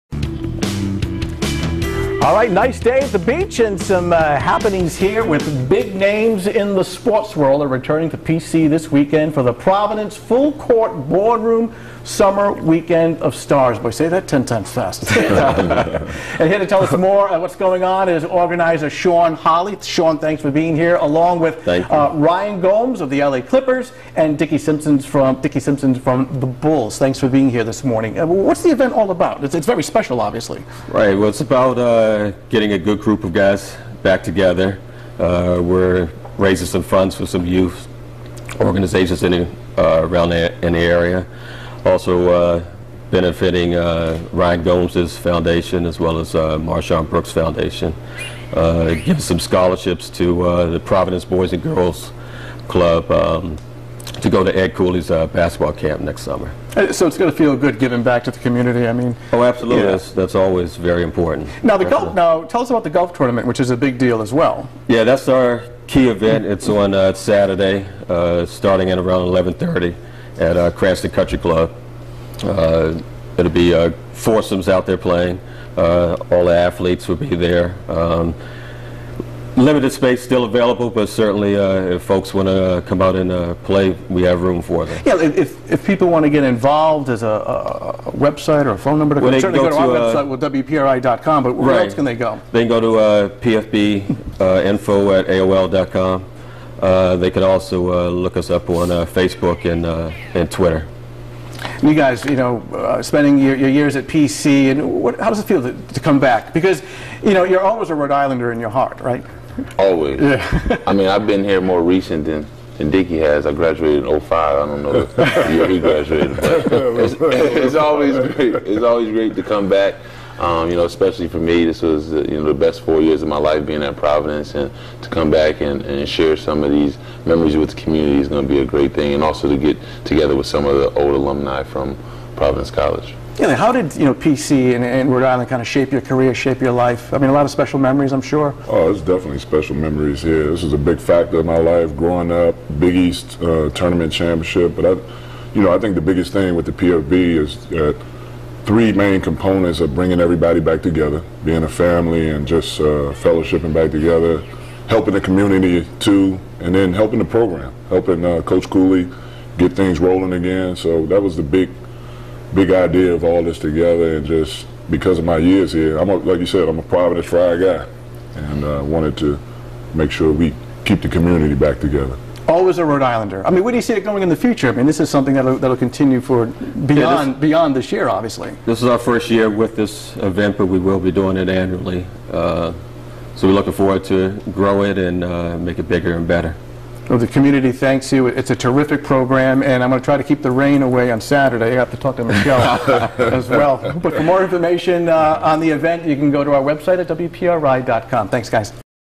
PC Weekend of Stars
The PROVIDENCE FULLCOURT BOARDROOM will be holding a first annual SUMMER WEEKEND OF STARS from August 9 to 12.